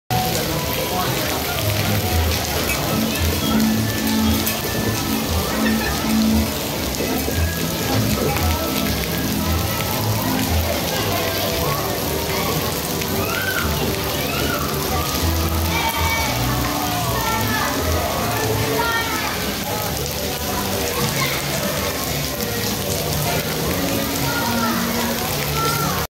The High in the Sky Seuss Trolly Train Ride! Emergency Stop Due to Weather
The High in the Sky Seuss Trolly Train Ride! at Universal's Islands of Adventure emergency stopping due to the rain in the area.